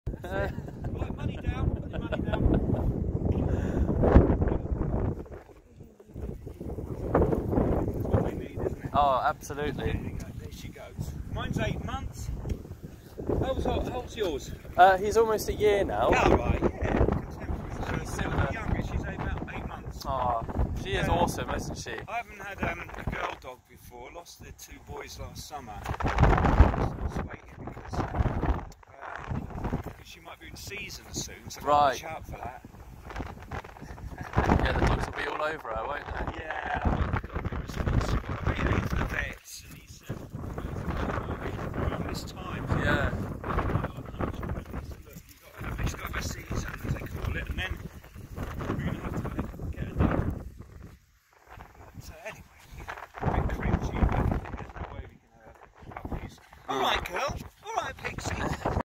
Whippet VS Saluki! Who Is Faster?
Today on our walk Freddie the whippet met Pixie the Saluki. They had a great time playing. I was quick enough to capture them on my phone 😂